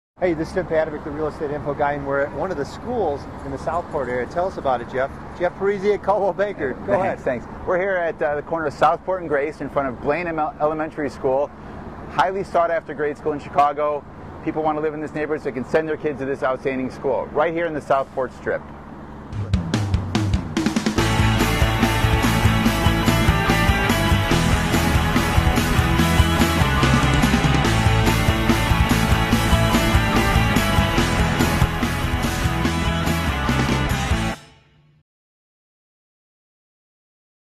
Blaine Elementary School- Southport
Jeff Parise of Coldwell Banker talks about Blaine Elementary School on Southport.  It appears that everyone wants their children to go to Blaine... Check out more